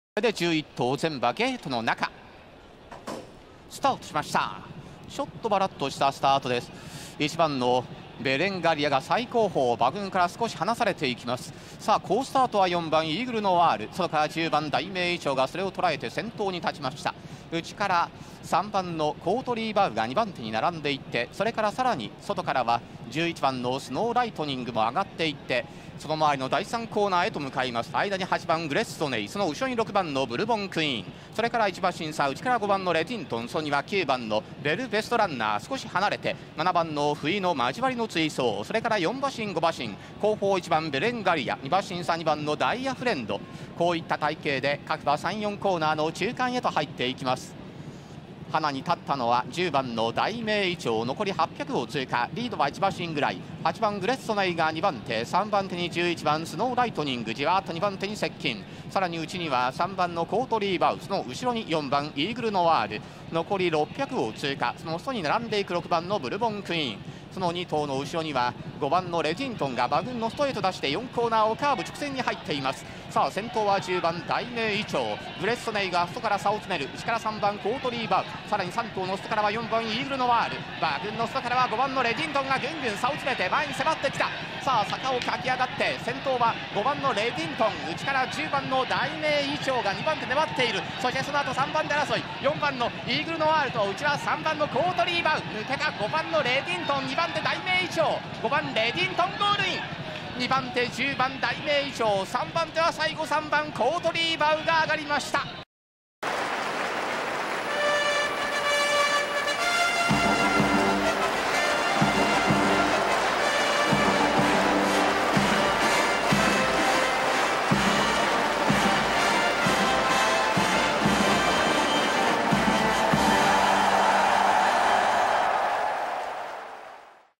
2歳新馬 芝1600m (右 外) レディントン 牡2 55.0 松若風馬 2023.6.18 出走馬：ダイメイイチョウ、コートリーバウ、イーグルノワール、ブルボンクイーン、フイノマジワリ他
2歳新馬
12:25発走 / 芝1600m (右 外) / 天候:曇 / 馬場:良
3回 阪神 6日目 サラ系２歳 新馬       (混)[指] 馬齢 11頭

レディントン
生年月日 2021年3月20日
調教師 杉山晴紀 (栗東)
馬主 長谷川祐司
生産者 藤澤純
産地 苫小牧市
セリ取引価格 -
獲得賞金 720万円 (中央)
通算成績 1戦1勝 [1-0-0-0]
主な勝鞍 23'2歳新馬
近親馬 ウインドジャマー、ハンソデバンド